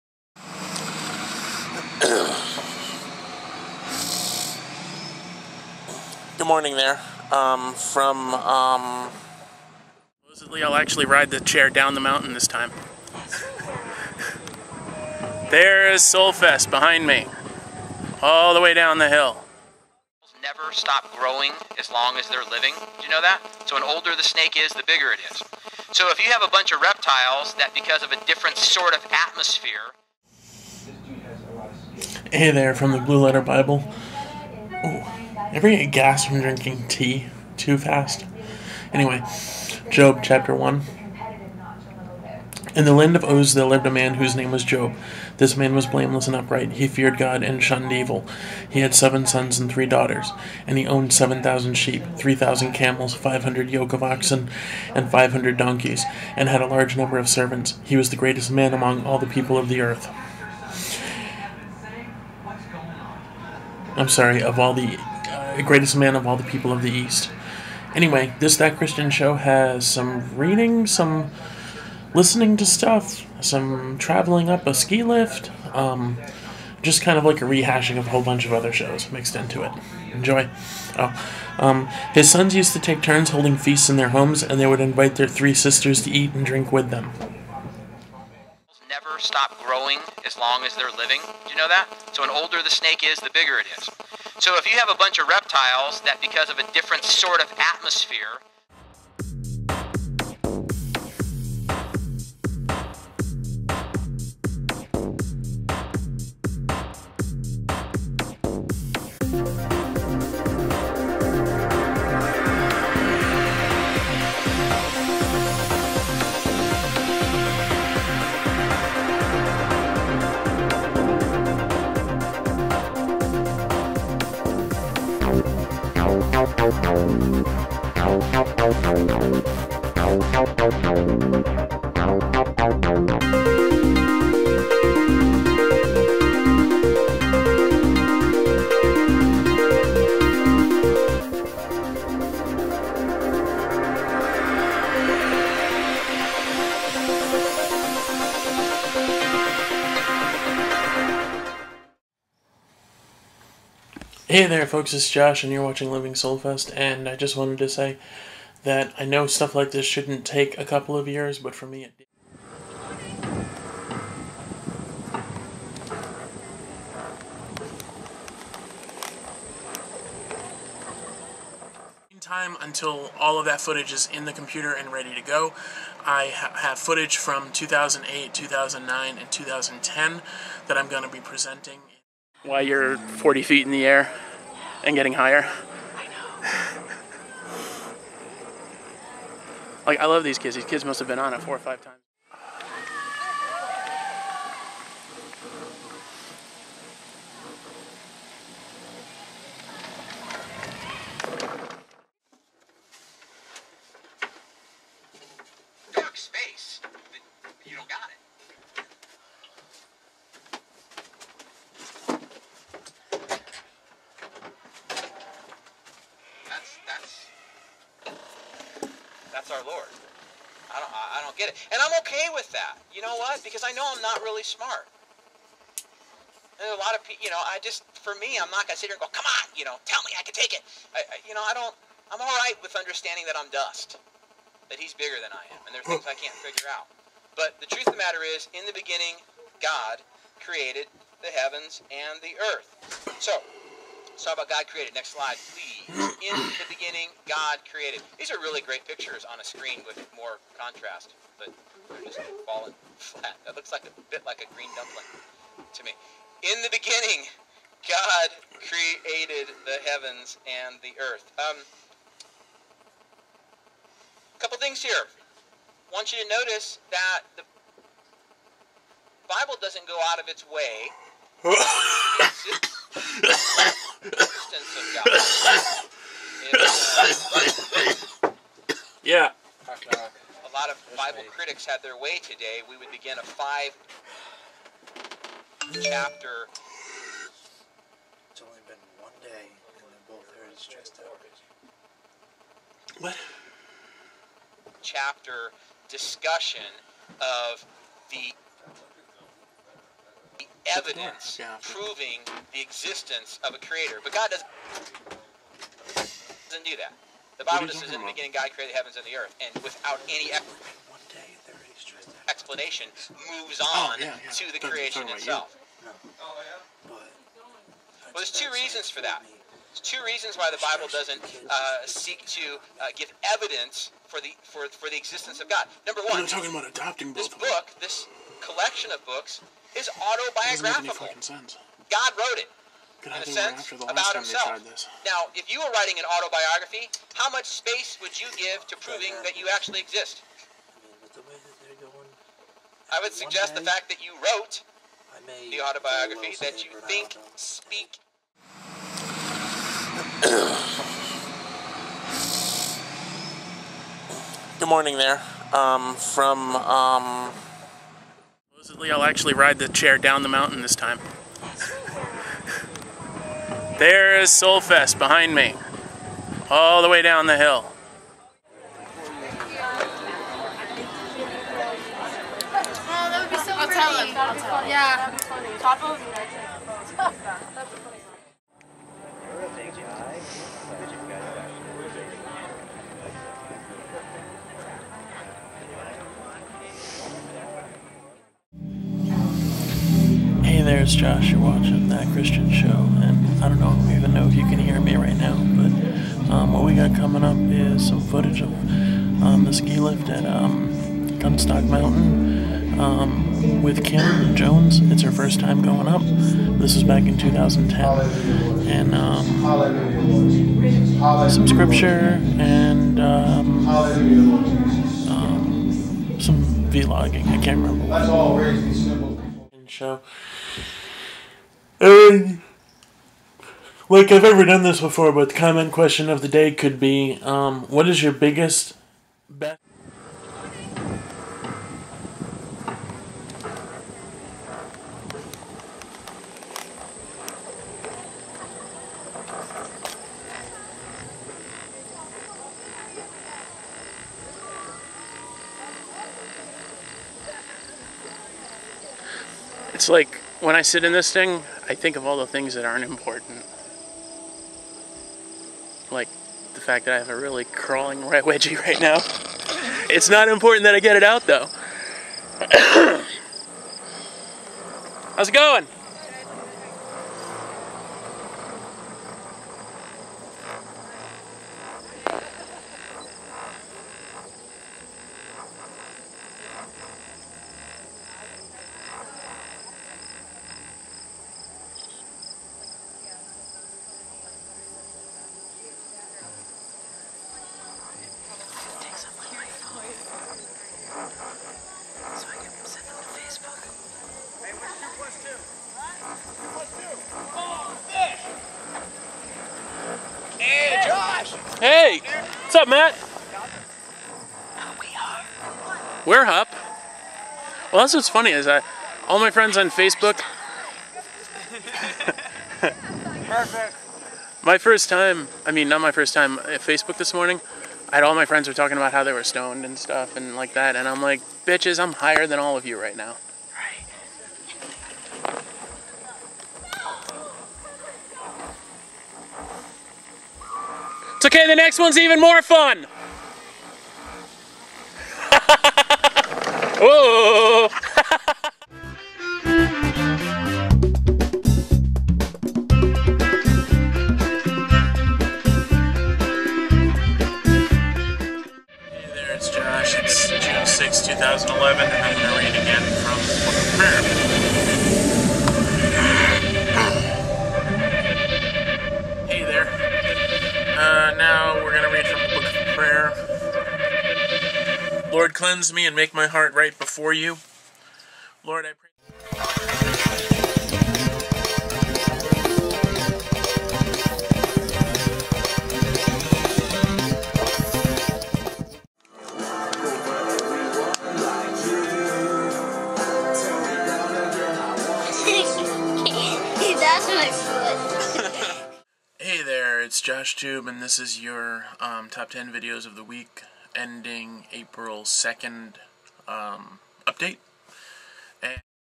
That Christian Show-March 6, 2012
rehashing old webisodes and some listening to a sermon by some guy...